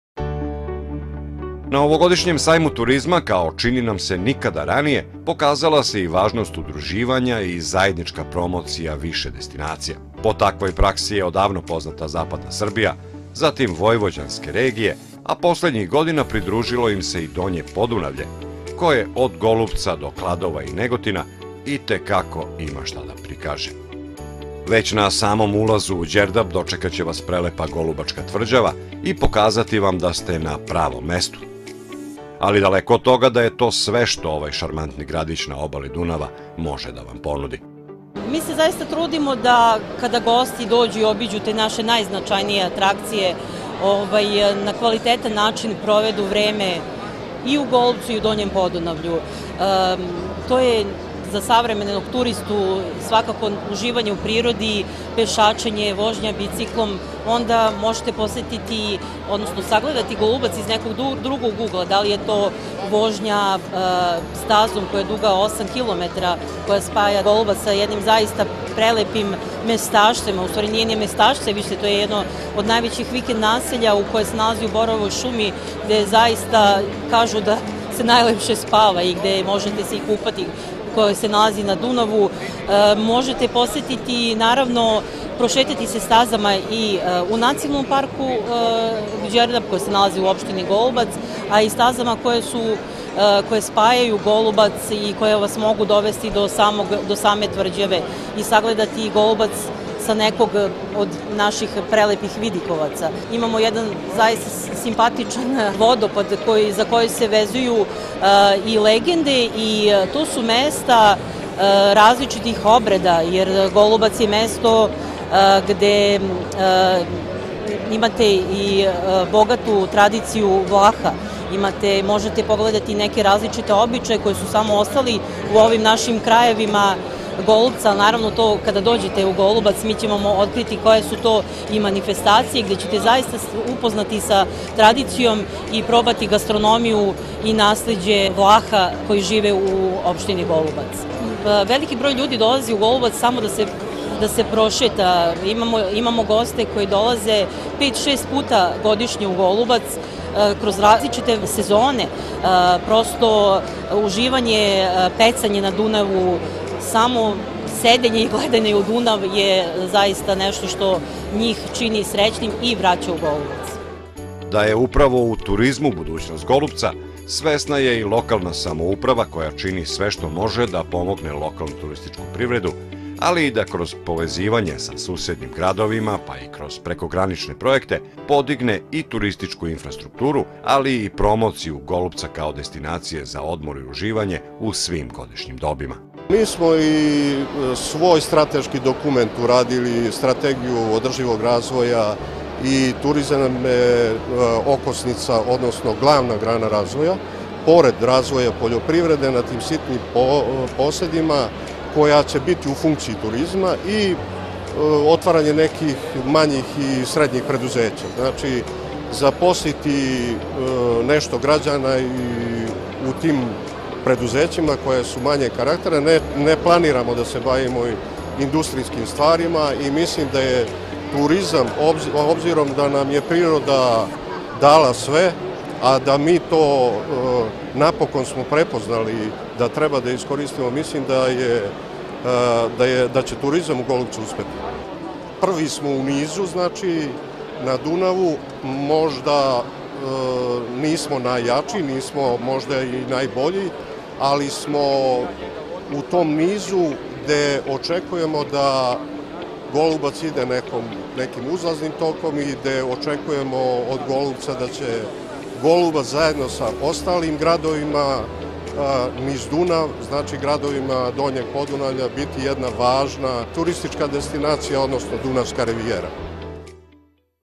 TO Golubac na Sajmu turizma u Beogradu 2023.
Promocija turističke ponude Opštine Golubac na 44. Međunarodnom sajmu turizma u Beogradu od 23. do 26. februara 2023. godine

Produkcija Matelli Media Niš